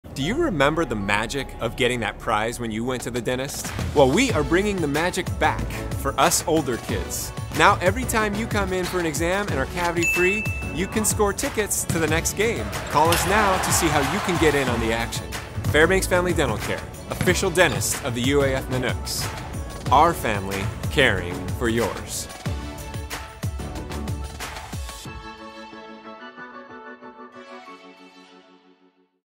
We are the Official Dentist of the UAF Nanooks!
Remember that magic of getting a prize as a child after going to the dentist? We are bringing that magic back to our Adult Cavity Free Club!

Stop by today to learn how we can help your score tickets to the next UAF home game!